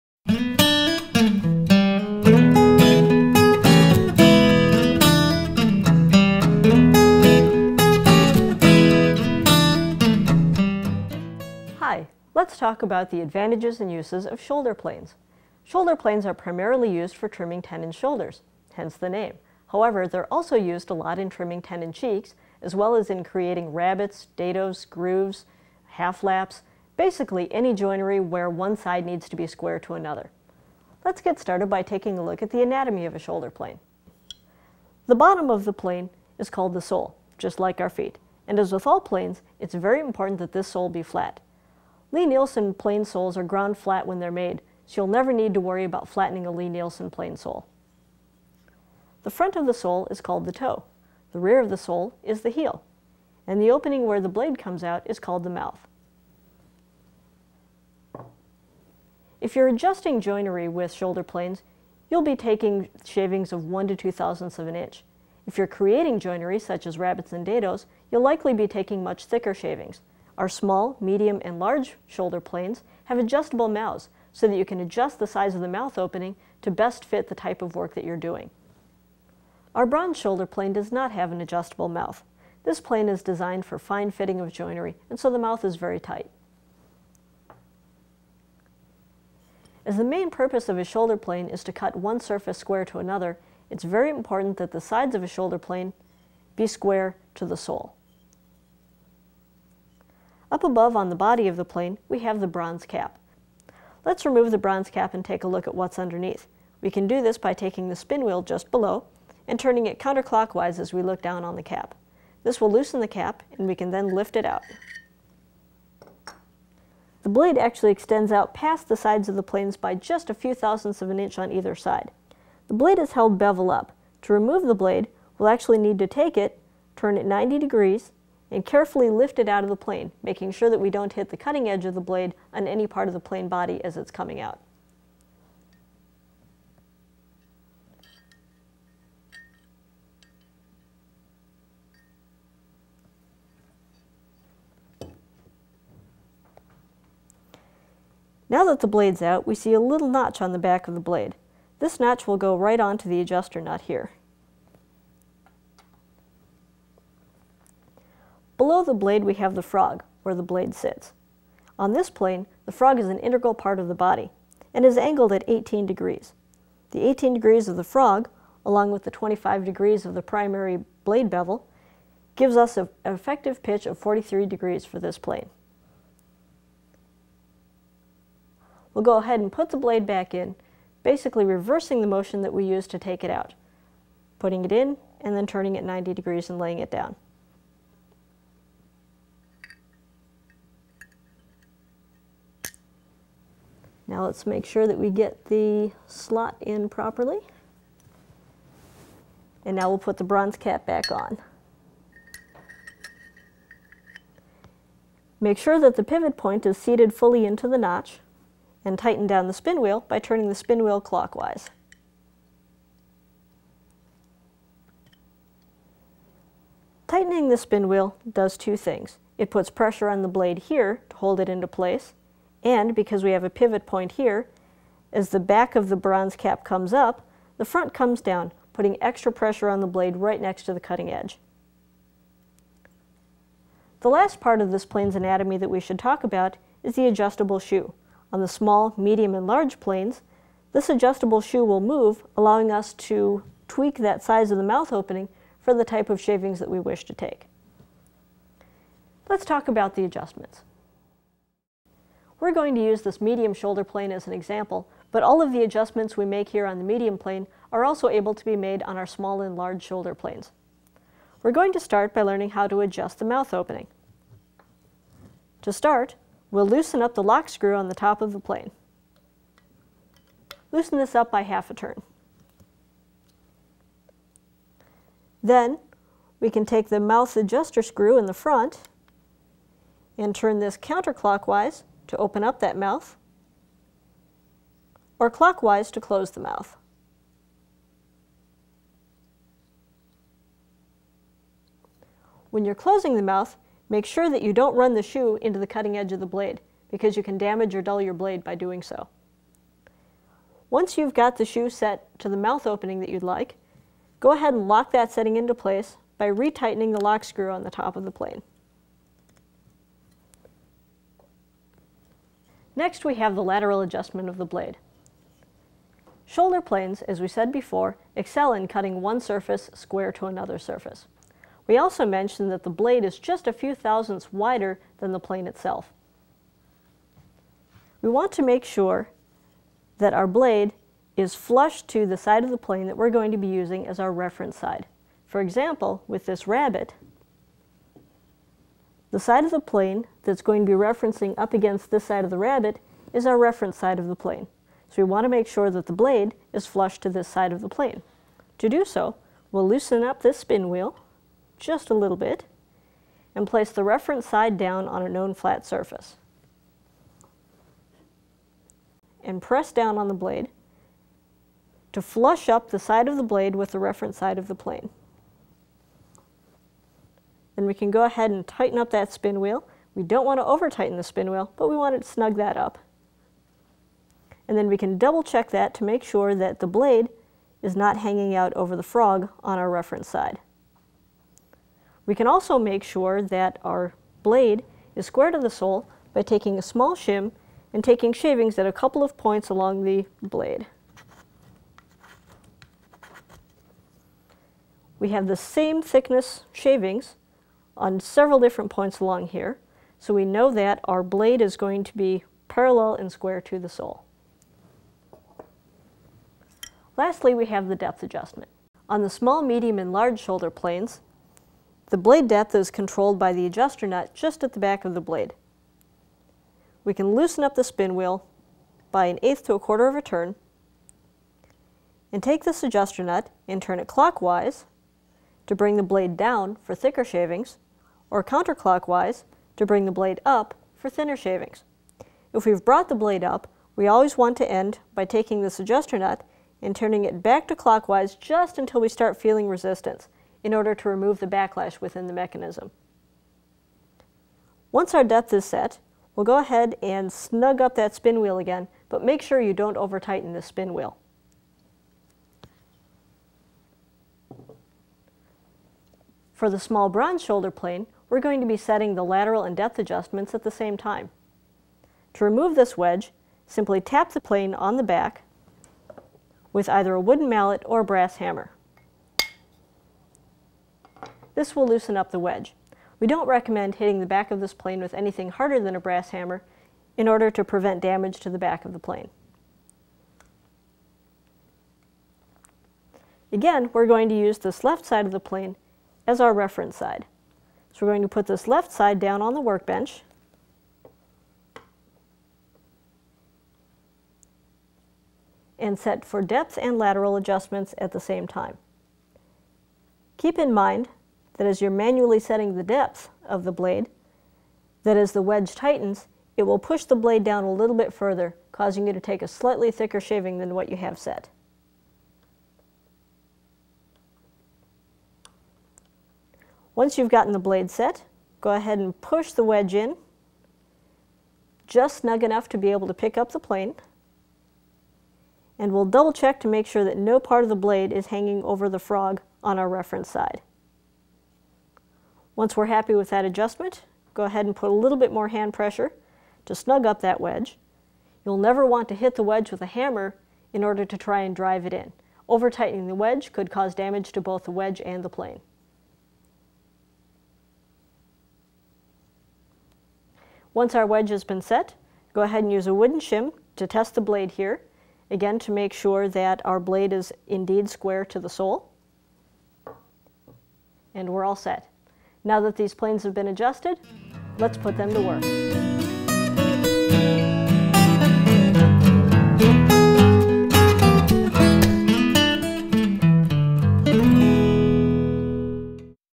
Shoulder Plane Part 1: Parts and Adjustment
Angie Kopacek breaks down the shoulder plane and shows how to properly adjust the tool to make precision cuts.